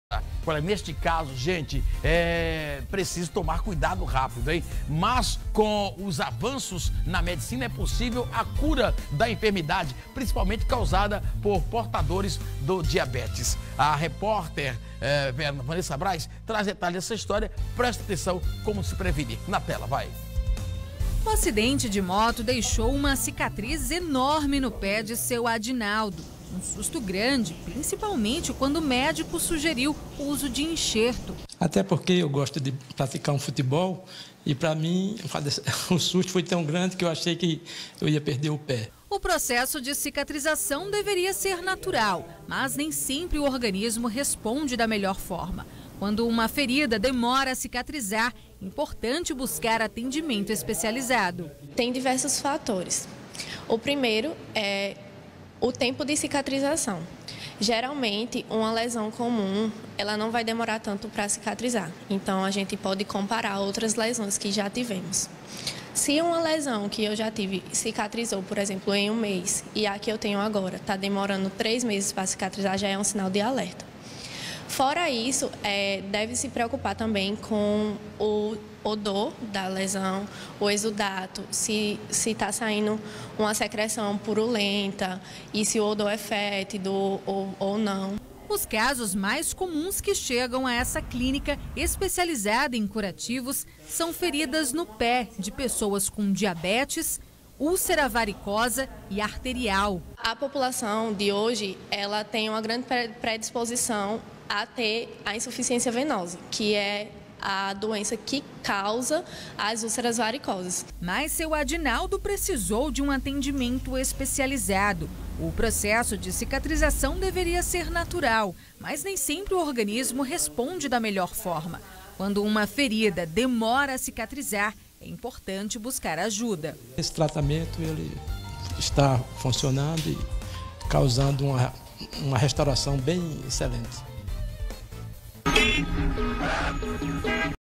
Feridas que demoram para cicatrizar pode ser um sinal da diabetes
Tambaú agora